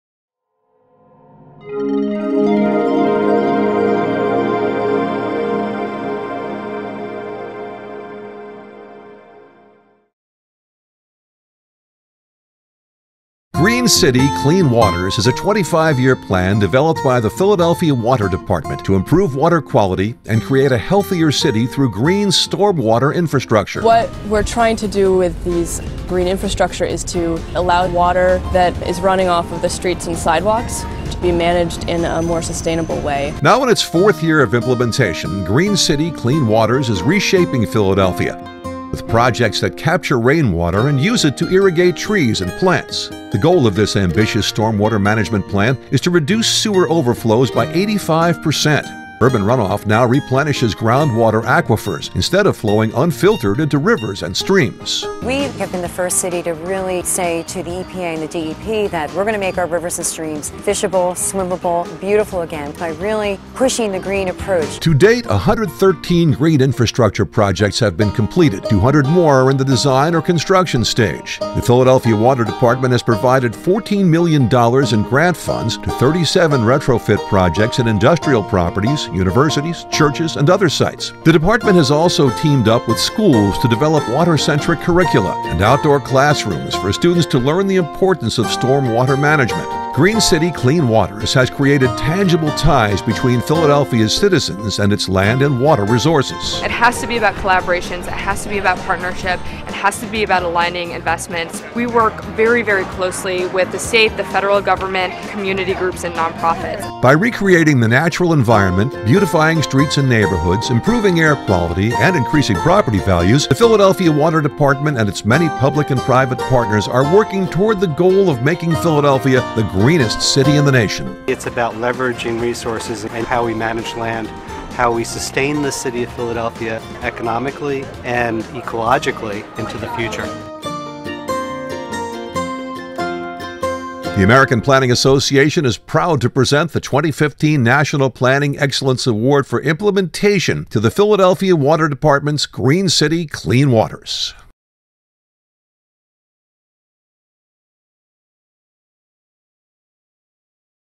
Green City, Clean Waters: Philadelphia's Green Stormwater Infrastructure Program
National Planning Excellence Award for Implementation

Green City, Clean Waters: Philadelphia's 21st Century Green Stormwater Infrastructure Program

Philadelphia, Pennsylvania

Hundreds of historic cities like Philadelphia experience combined sewer overflows during heavy rainstorms. Philadelphia Mayor Michael Nutter gave the green light to explore reverting from "gray," or traditional stormwater management infrastructure, to "green" or sustainable infrastructure that mimics the natural water cycle.

In 2011, Philadelphia reached an agreement with the Pennsylvania Department of Environmental Protection and the U.S. EPA to explore a largely green stormwater management infrastructure plan to reduce sewage overflows. As a result, an ambitious and comprehensive attempt to overhaul stormwater infrastructure was developed.

Green City, Clean Waters (GCCW) has not only made Philadelphia the first city in the U.S. to meet both state and federal water quality mandates through green interventions, but it will also save the city an estimated $6.5 billion in construction costs over building new pipes.